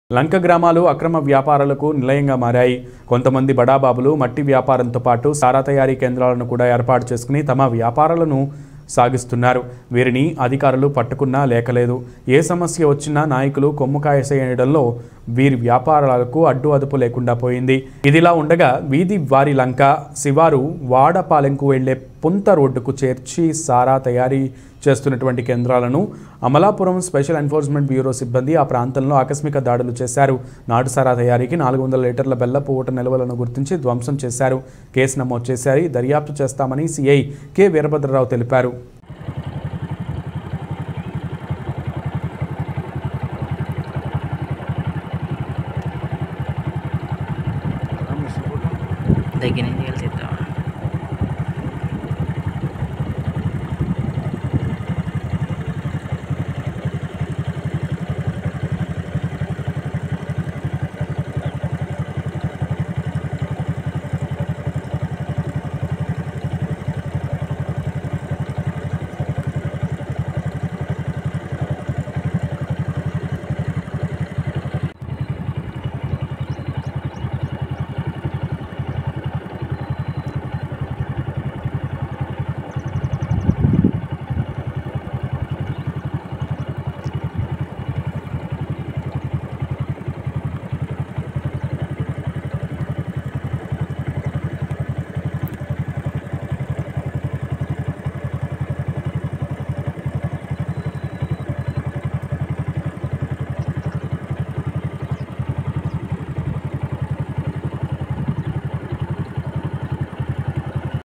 ABC NEWS TELUGU// అక్రమ వ్యాపారాలకు నిలయంగా మారిన లంక గ్రామాలు
కపిలేశ్వరపురం
 15/12/2022
రిపోర్టర్ :పి. శ్రీనివాసు

యాంకర్ పార్ట్ :

         లంక గ్రామాలు అక్రమ వ్యాపారాలకు నిలయంగా మారాయి. కొంతమంది బడాబాబులు మట్టి వ్యాపారంతో పాటు, సారా తయారీ కేంద్రాలను కూడా ఏర్పాటు చేసుకుని తమ వ్యాపారాలను సాగిస్తున్నారు.
            వీరిని అధికారులు పట్టుకున్నా లెక్కలేదు, ఏ సమస్య వచ్చినా నాయకులు కొమ్ము కాసేయడంతో వీరి వ్యాపారాలకు అడ్డు అదుపు లేకుండా పోయింది.
          డా బి ఆర్ అంబేద్కర్ కోనసీమ జిల్లా  కపిలేశ్వరపురం మండలం కేదార్లంకా శివారు బొల్లంక పరిసర ప్రాంతాల్లో భారీగా అక్రమ మట్టి నిల్వలను ఏర్పాటు చేశారు, బట్టీలకు తరలించేందుకు సన్నాహాలు చేస్తున్నారు, స్థానికులు ఈ విషయాన్ని వీఆర్వో దృష్టికి తీసుకురావడంతో వాటిని పరిశీలించారు, స్థానిక తాసిల్దార్ దృష్టికి ఈ విషయాన్ని తెలియపరుస్తానని వీఆర్వో తెలిపారు,

యాంకర్ వాయిస్ :

 ఇదిలాగ ఉండగా వీధి వారిలంక శివారు వాడపాలెంకు వెళ్లే పుంత రోడ్డుకు చేర్చి సారా తయారి చేస్తున్న కేంద్రాలను అమలాపురం స్పెషల్ ఎన్ఫోర్స్మెంట్ బ్యూరో సిబ్బంది ఆ ప్రాంతంలో ఆకస్మిక దాడులు నిర్వహించారు నాటు సారా తయారికి 400 లీటర్ల  బెల్లపు ఊట నిల్వలను గుర్తించి ధ్వంసం చేశారు. దీనిపై కేసు నమోదు చేసామని సీఐ కె.వీరభద్రరావు తెలిపారు.